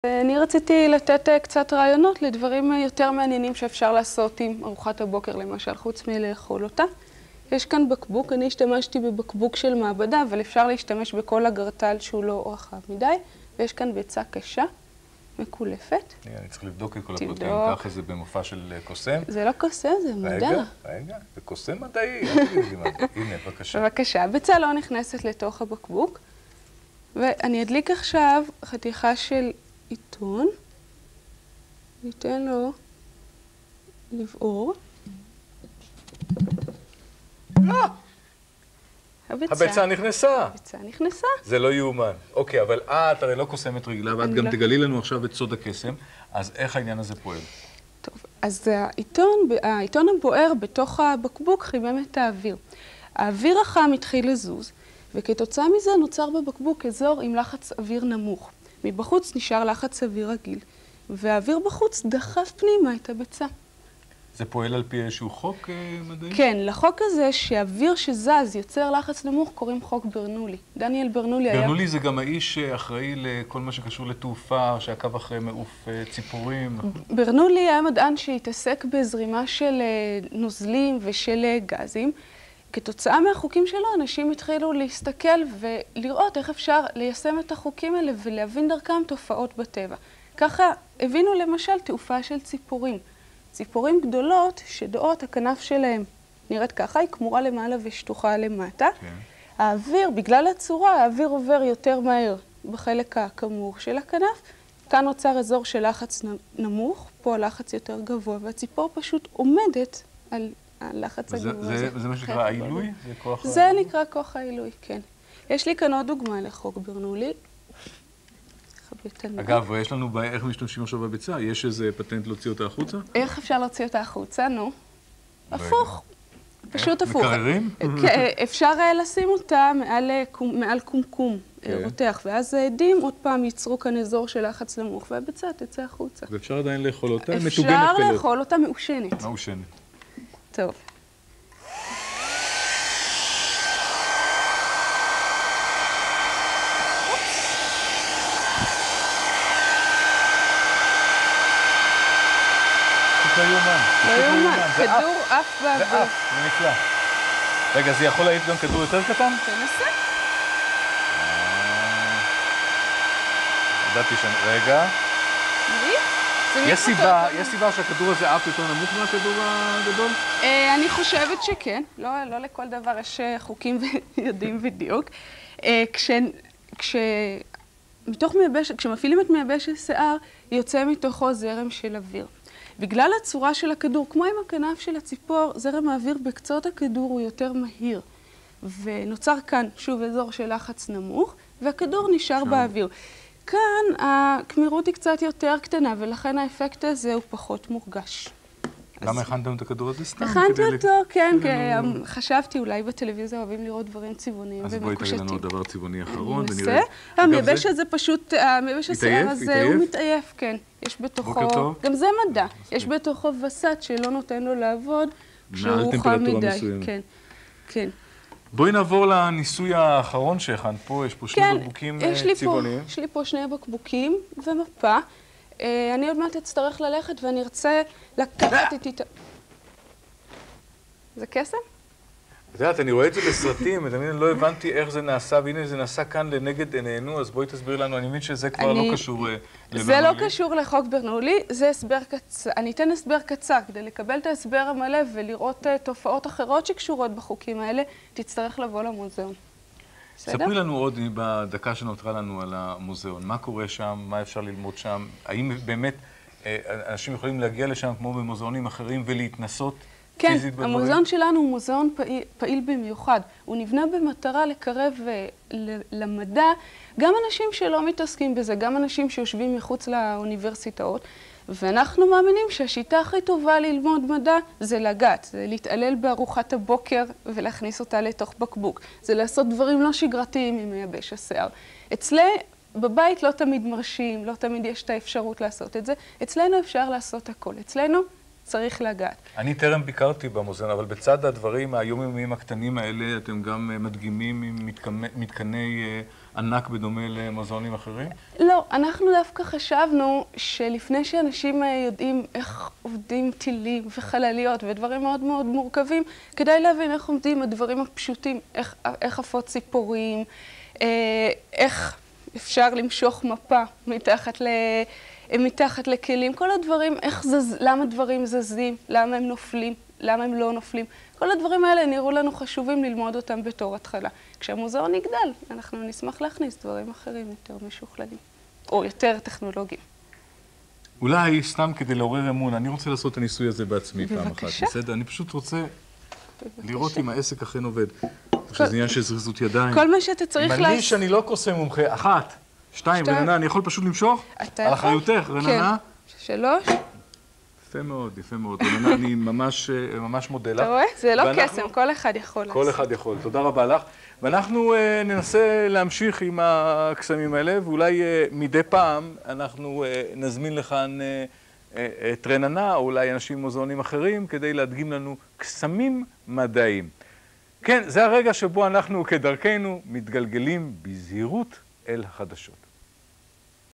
פינה על מוזיאון המדע בתכנית טלויזיה
כל מה שאנחנו יודעים על התכנית שהיא שודרה בתחילת שנות התשעים והיא בהנחיית דליק ווליניץ. אם יש לכם מידע נוסף על התכנית, נשמח לקרוא אותו בתגובות.